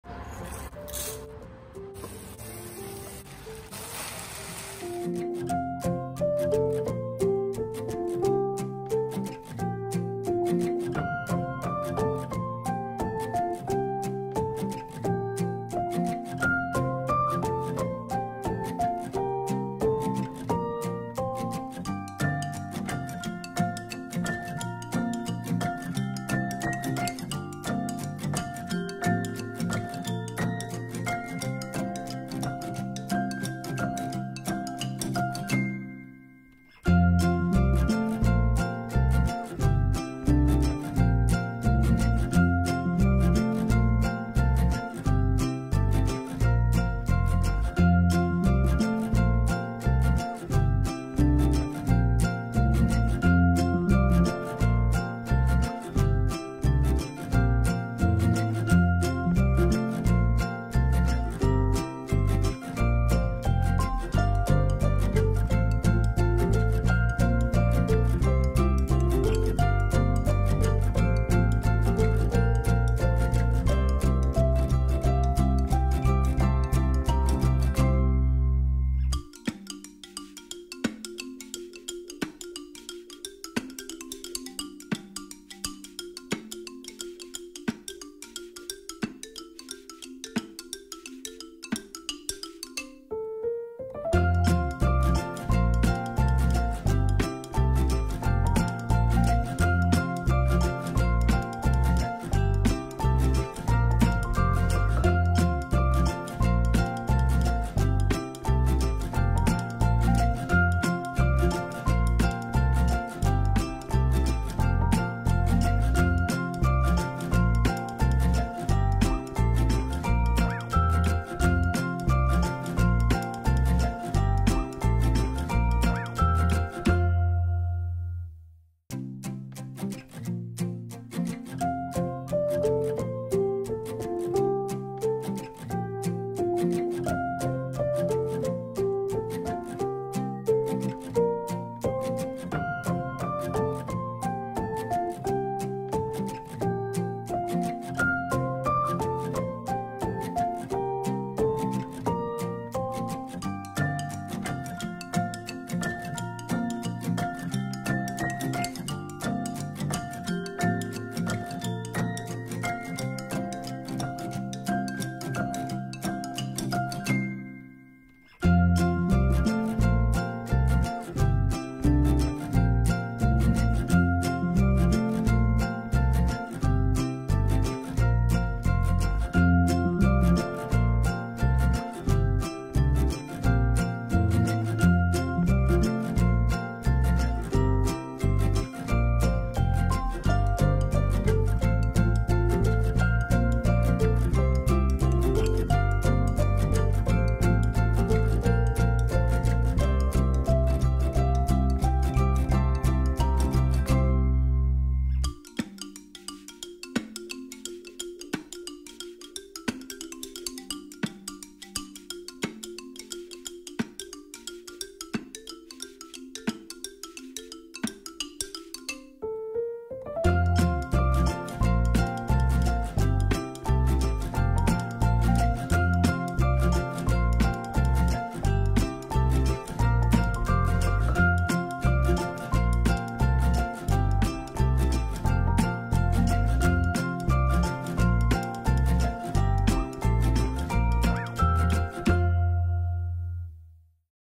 QUICK AND EASY TUNA PASTA RECIPE
HOW TO COOK SPICY TUNA PASTA | SIMPLE AND EASY RECIPE

Ingredients:
300g pasta
1 can century tuna
3 cloves garlic
1 onion
2 tbsp butter or cooking oil
3 tbsp olive oil
Chili flakes (optional)